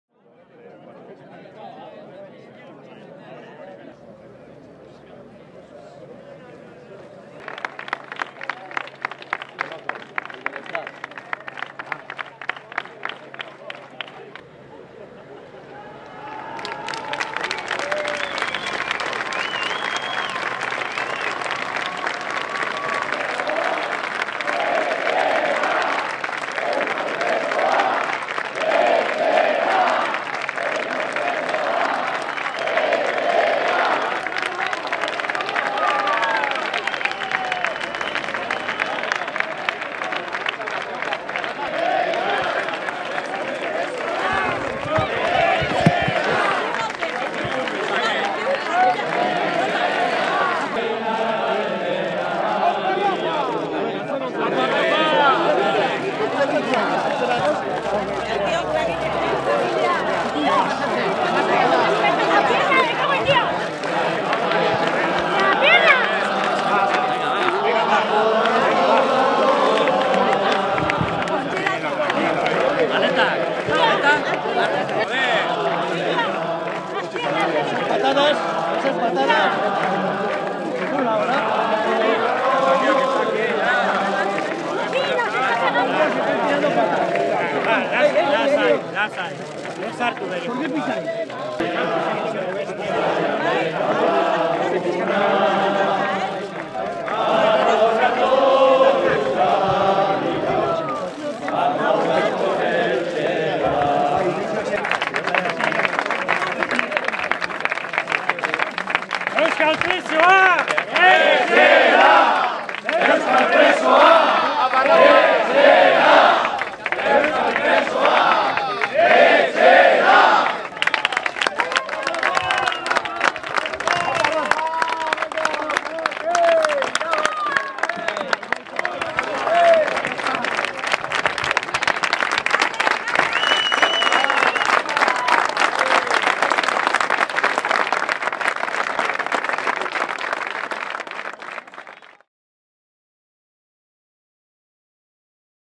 Rafa Diezen irteera espetxetik
Espetxe zigorra osorik beteta, LABeko idazkari nagusi ohia gaur goizean irten da Santoñako (Kantabria, Espainia) kartzelatik.